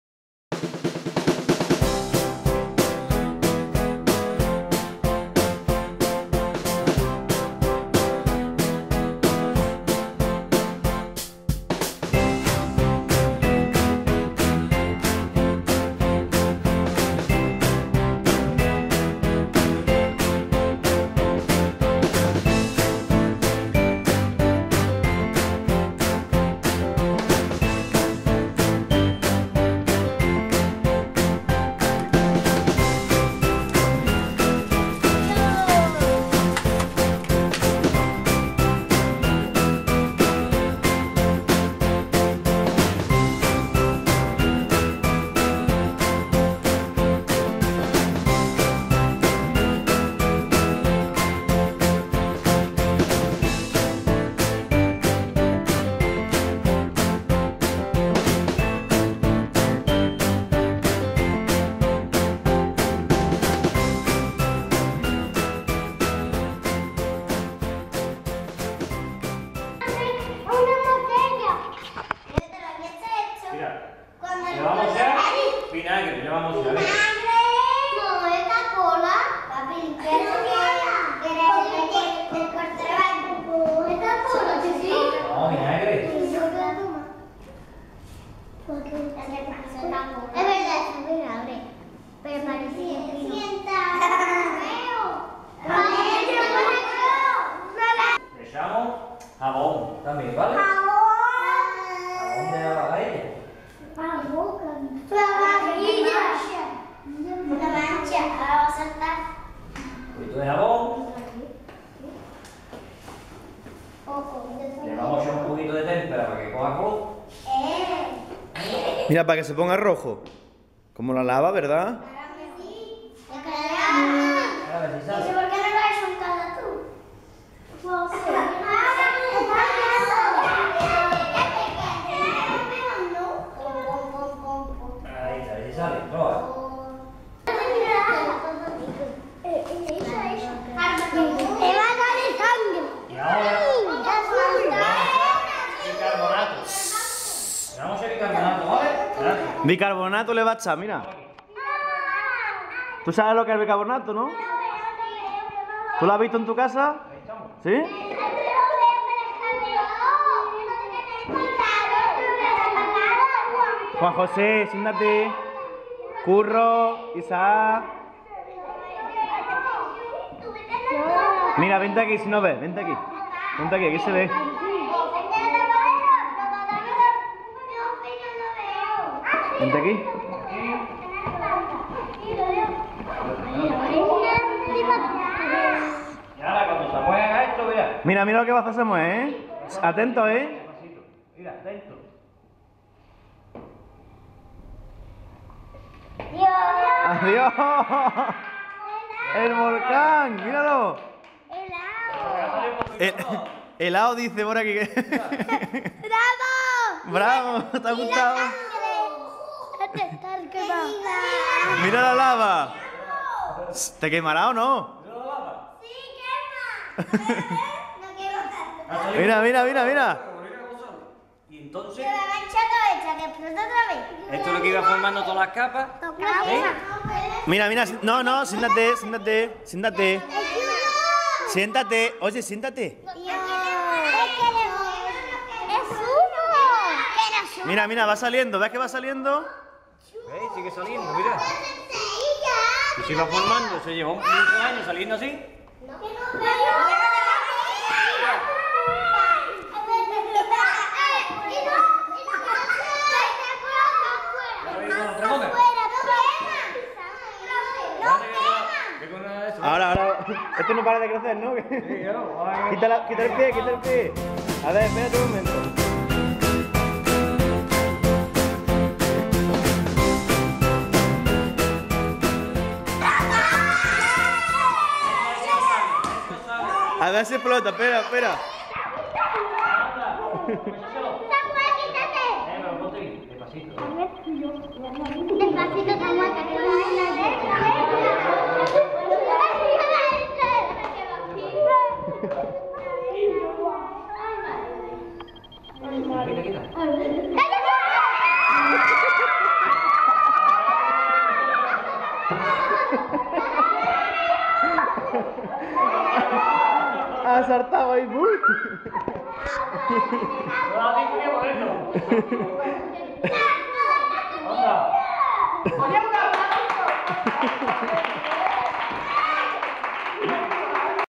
¿Cómo hacer un volcán?
Hoy os traemos esta manualidad. ¿Quieres hacer un volcán? Samuel y su papá nos explican en clase cómo hacerlo, un auténtico volcán que entra en erupción.

Si te ha gustado dale a "me gusta".